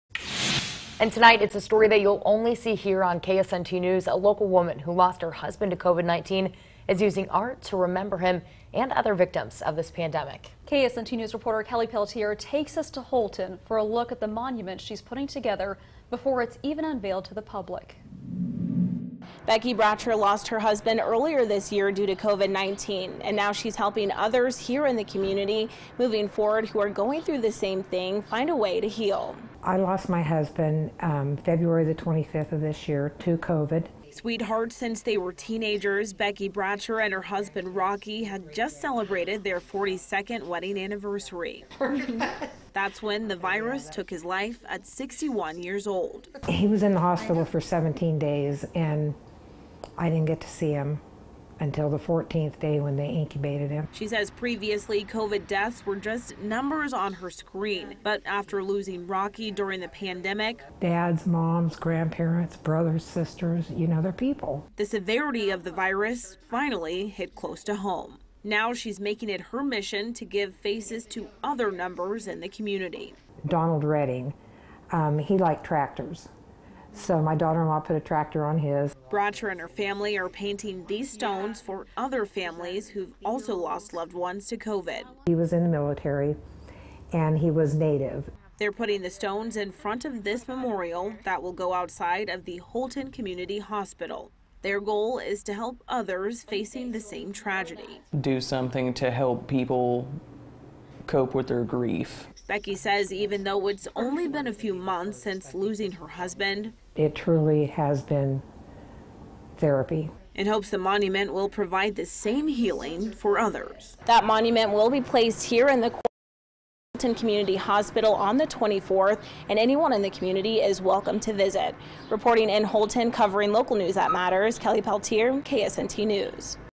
With husband lost to COVID-19, Kansas widow making monument for him & other victims
A Holton widow is using art to memorialize her husband and other victims lost in the COVID-19 pandemic.